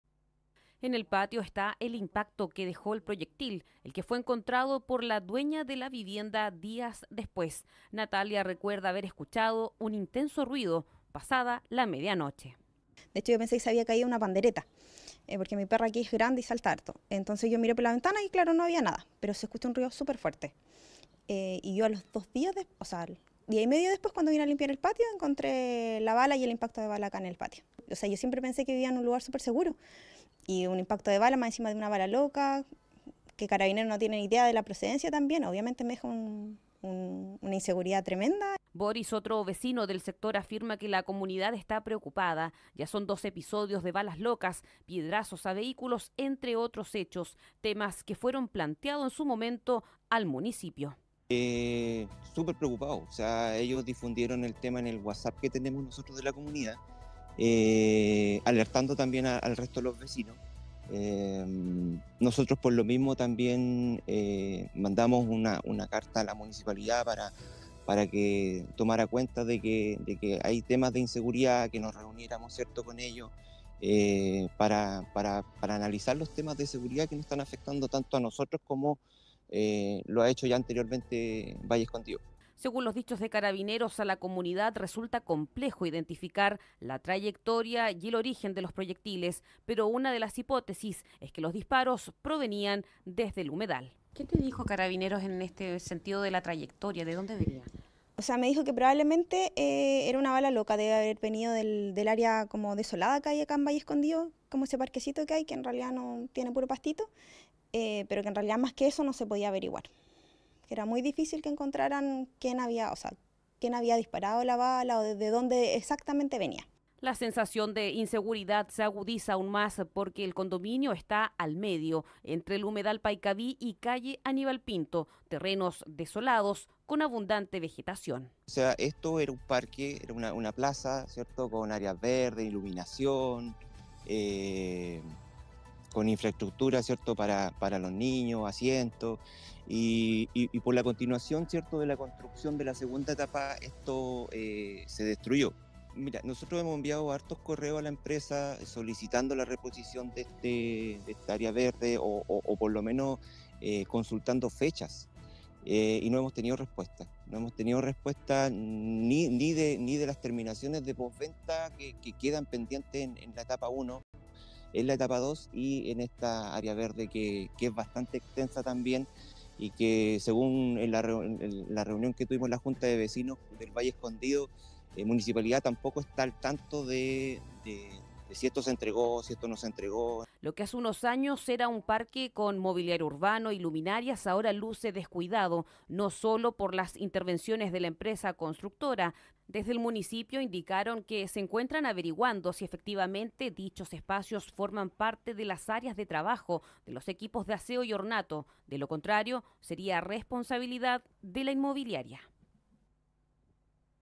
Vecinos del condominio Portal Aníbal Pinto denuncian balaceras que han afectado su seguridad
Vecinos del condominio Portal Aníbal Pinto están preocupados, ya que el pasado mes de diciembre al menos dos viviendas han sido afectadas por el impacto de armas de fuego.

Aunque los proyectiles no causaron daño a personas, sí les han generado una sensación de inseguridad.

Canal 9 Biobio TV

👤 Facebook: Canal 9 Biobio TV
 𝕏  Twitter: @Canal9BiobioTV